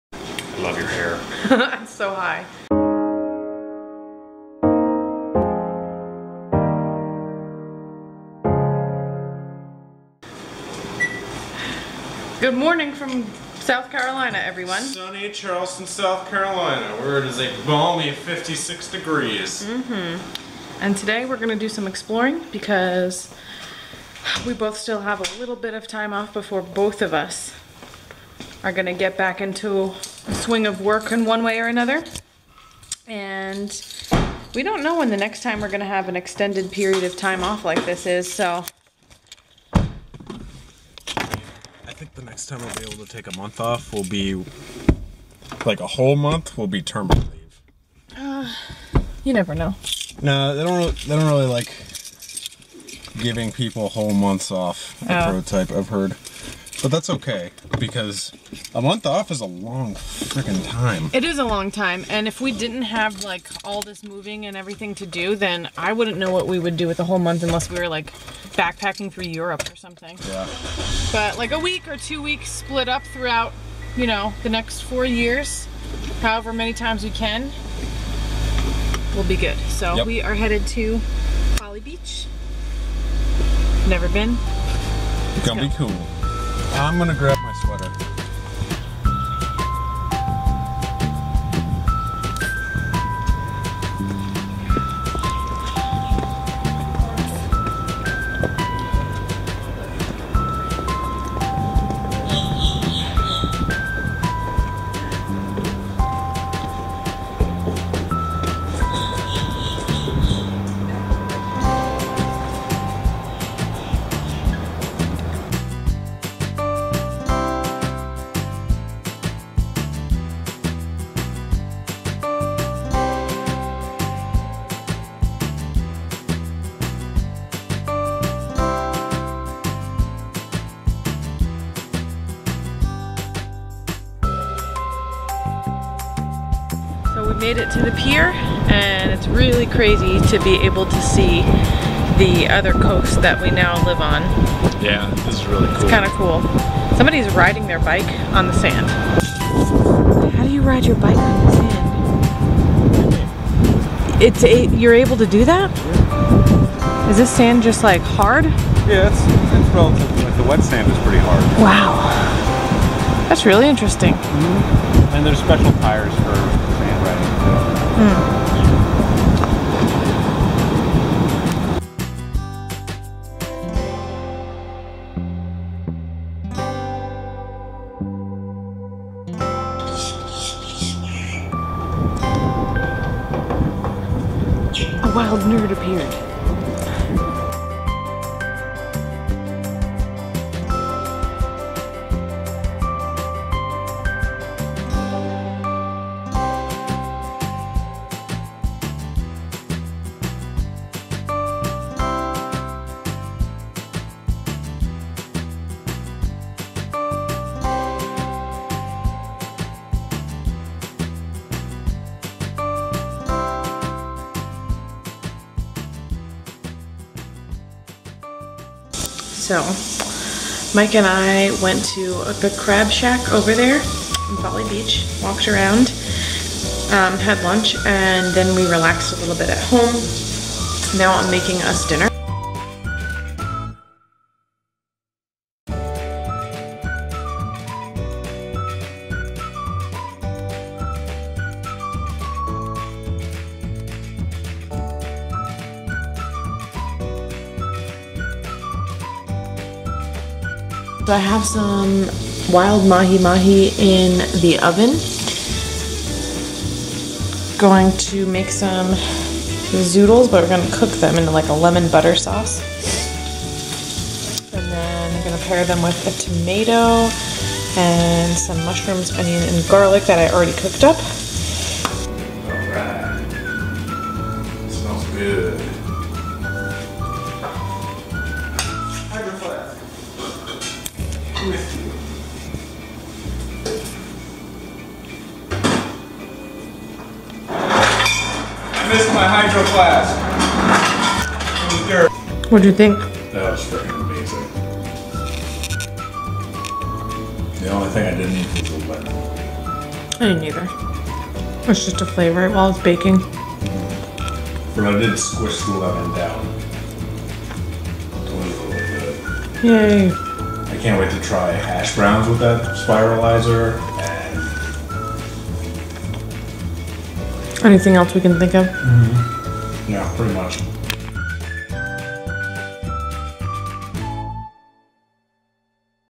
EXPLORING FOLLY BEACH | South Carolina Vlog
In this vlog, Mike and I explore a bit of Folly Beach. This was filmed before the both of us went back to work. Being there made me feel grateful that we have so much to explore and do in this new state of ours!

POSHMARK: USE CODE JXBGG For a $10 credit on your first sale!

UBER: send friends a free ride and get one worth $20 CODE: valerie549